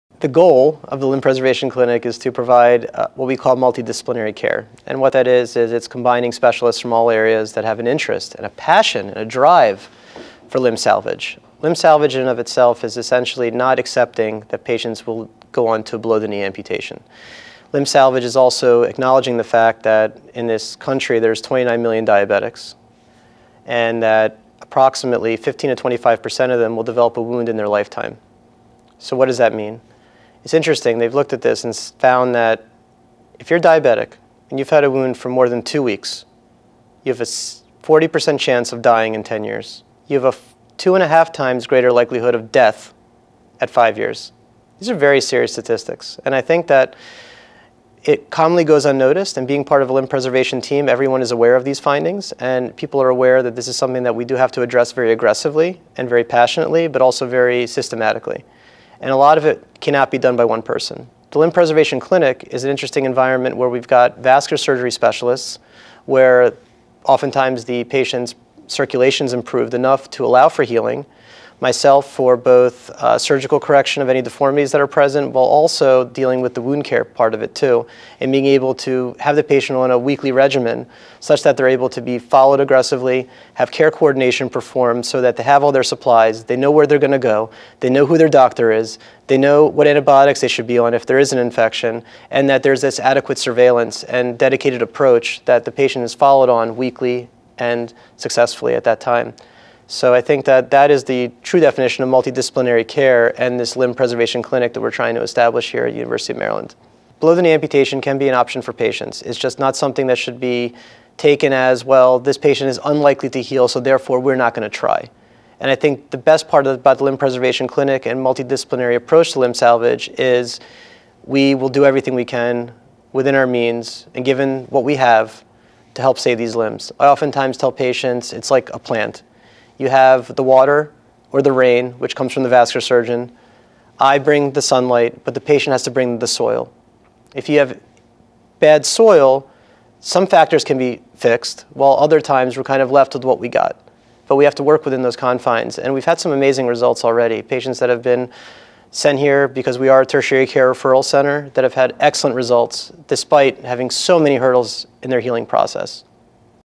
Limb Preservation with Jacob Wynes, DPM
The goal of UMMC's limb preservation clinic is to bring experts from different disciplines to give patients complete care. Jacob Wynes, DPM, leads the limb preservation team.